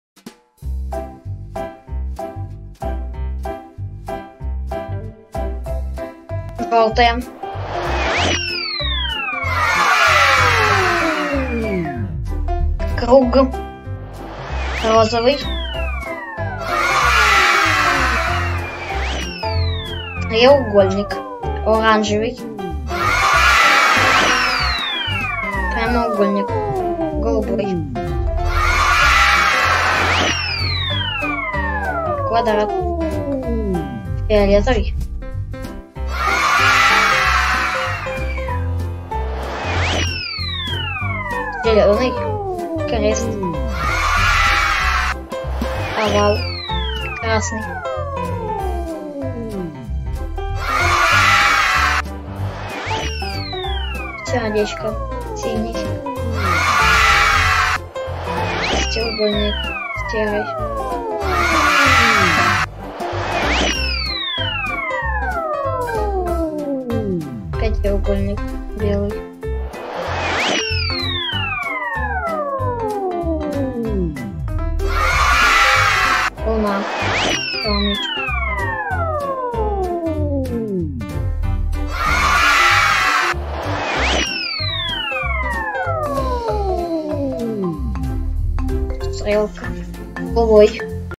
Мультик для малышей - Учим цвета и геометрические фигуры
Обучающий мультфильм про машинку, в которой едут разноцветные фигуры, мультик расскажет про цвета и формы!